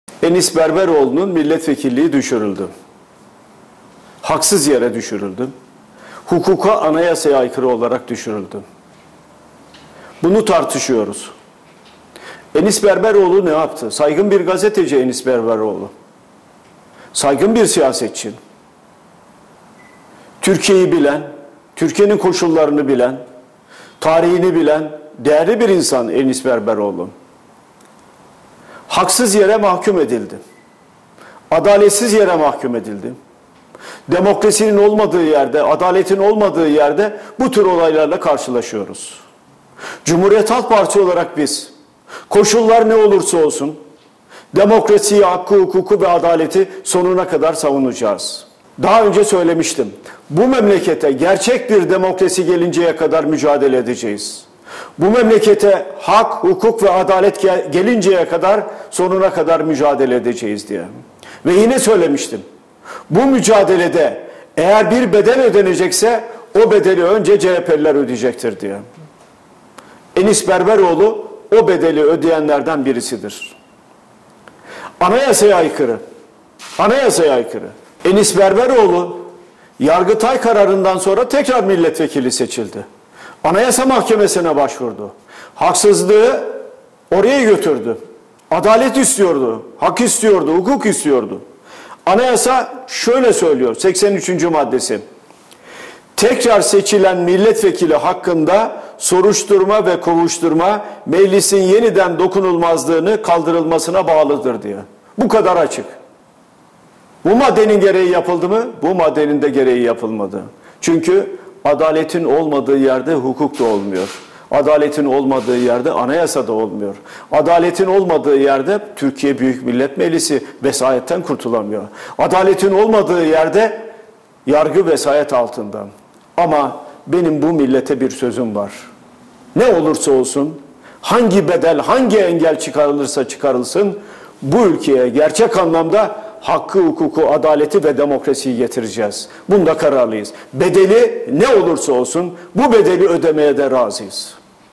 CHP LİDERİ KILIÇDAROĞLU'NDAN ENİS BERBEROĞLU AÇIKLAMASI
Milletimize sözümüz var!
Ne olursa olsun, hangi bedel hangi engel çıkarılırsa çıkarılsın; bu ülkeye gerçek anlamda hakkı, hukuku, adaleti ve demokrasiyi getireceğiz.